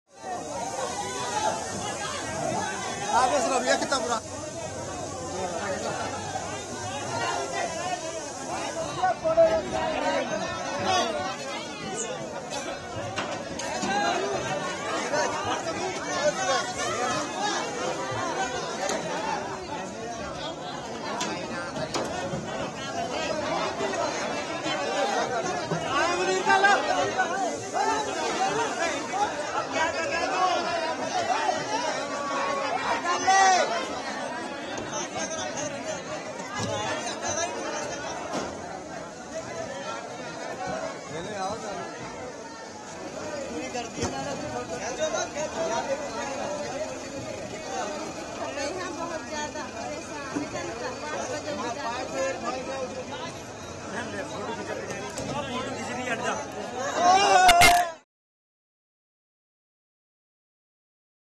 Delhi Water Crisis: Severe water crisis grapples Delhi’s Okhla, locals struggle
Severe heatwave conditions in Delhi have thrown normal life out of gear. Due to the record-high temperature, there has been a rapid surge in water demand. Locals from the Okhla area are facing an acute water crisis. They were seen standing in queues and waiting for their turn to get water from tankers. Meanwhile, the maximum temperature reported in Delhi was 47 Degrees Celsius.